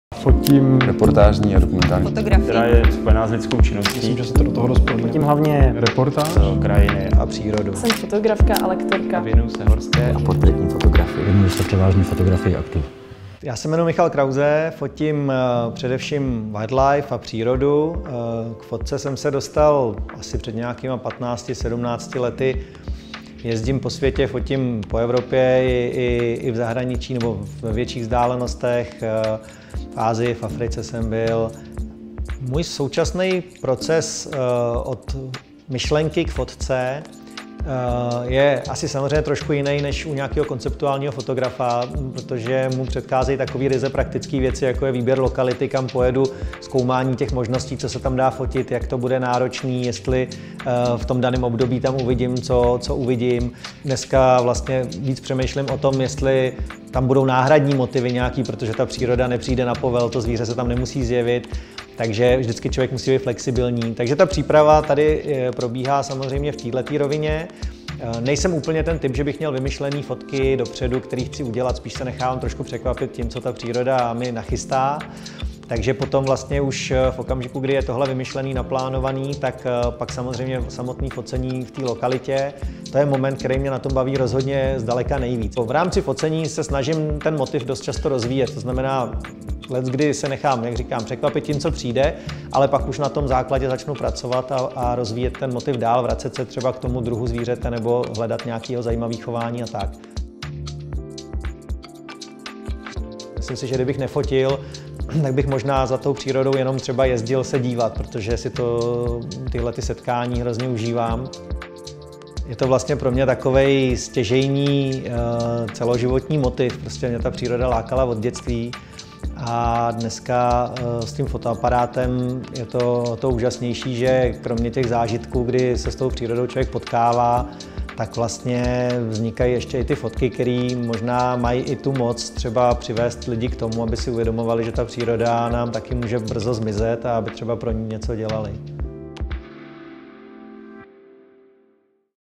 About: Michal Krause
Michal Krause fotografuje divokou přírodu po celém světě. Má rád minimalismus a jednoduchost ve fotografii, technice i životě. Necestuje za konkrétním snímkem, naopak se rád nechává přírodou překvapit. Lépe se cítí v roli pozorovatele než hybatele, který scénu nějakým způsobem utváří, a nejúžasnější jsou pro něj ty momenty, kdy jej zvířata akceptují jako přirozenou součást svého okolí.